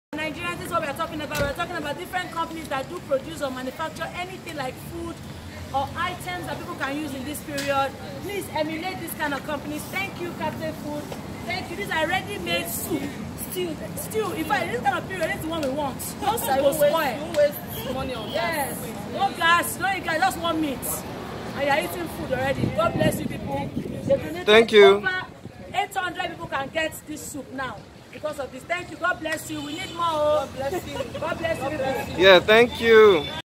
Charity Collaboration with Kaffy!
We collaborated with the wonderful award-winning dancer, Kaffy to distribute sachets of ReadyStews to those in need.
No pandemic should separate people from tasty, healthy food!
We got you! 🙏🏽